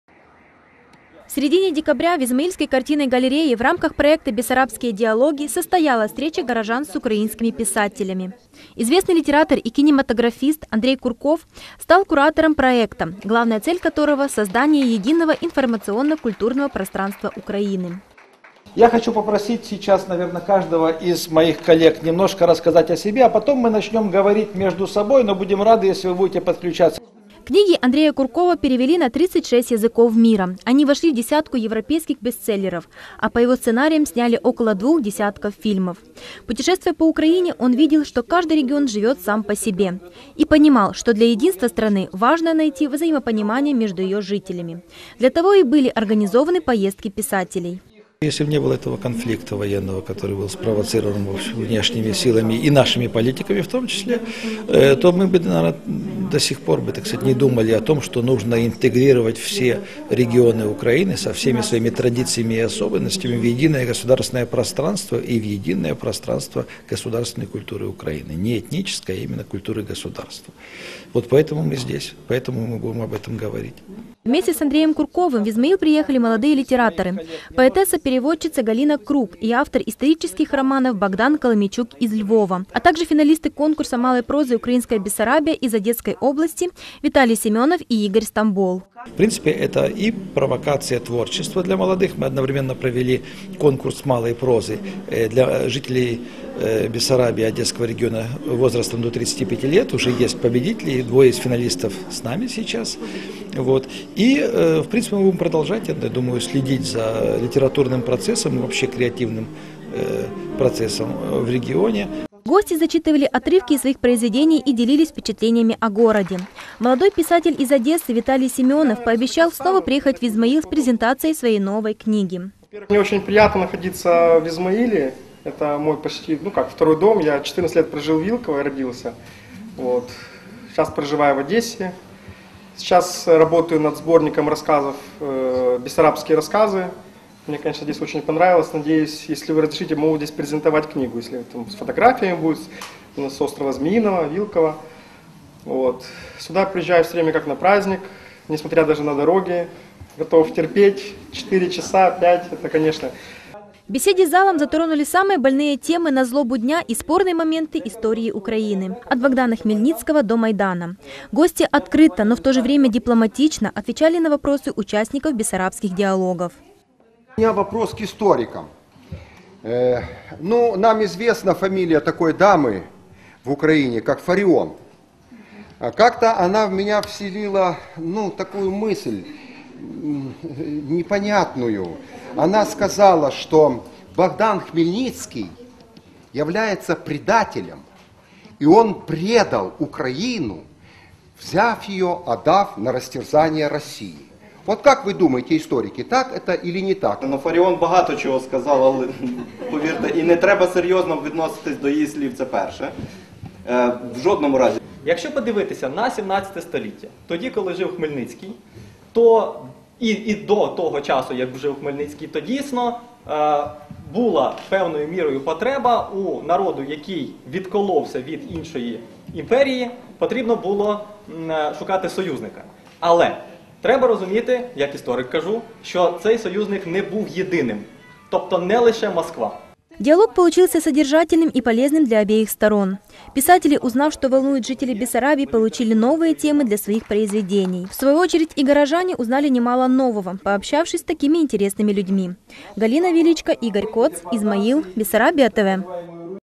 «Бессарабские диалоги» в Измаиле
В конце осени в Одесской области прошла презентация нового культурно-художественного проекта "Бессарабские диалоги", инициированного Министерством культуры и известным украинским писателем Андреем Курковым. Не обошли стороной и столицу Придунавья – подробности смотрите в слеующем сюжете